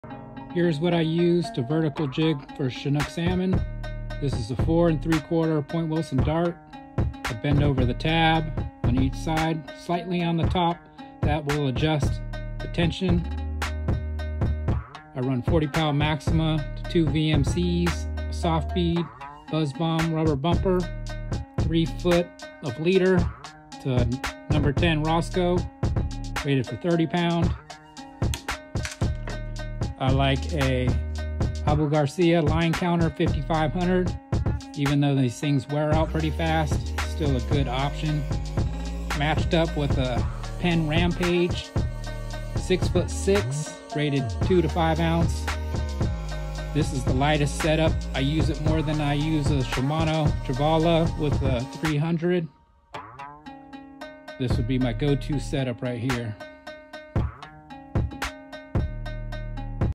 Pro Tip : Catch More Salmon Vertical Jigging : Chinook Salmon Fishing South Puget Sound
Chinook salmon fishing
40 lb maxima 4/0 vmc #10 rosco 40lb power pro
4 3/4 candle fish point wilson dart buzz Bomb rubber stopers soft bead 6’6” Penn rampage jigging abu Garcia 5500 line counter you must have a line counter no exception. You’l never catch fish in the water column with out this. You must also lift and drop tour rod tip immediately to get a free falling jig this is the single most important factor over color. Fish react - respond to the falling bait. If you know salmon behavior then you know how a dropping bait gets more bites.